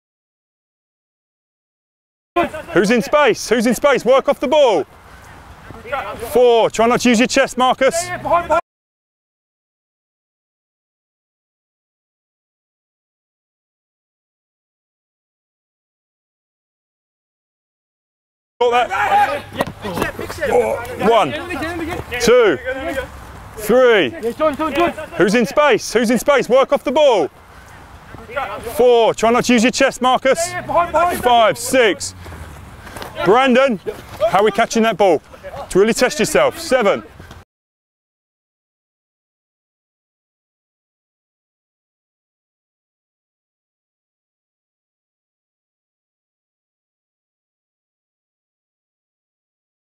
Offloading Grip Game | Harlequins Academy | Featuring Marcus Smith
Anyone pick out a young Marcus Smith? Check out this offloading drill from Harlequins Academy, working on passing and receive with one hand to develop a vice like grip. Take a look at the full rugby drill with description and coaching points

Rugby Coaching Made Easy. Over 1100 Rugby Drills, Techniques & Rugby Coaching Plans plus powerful tools to help you become a better coach.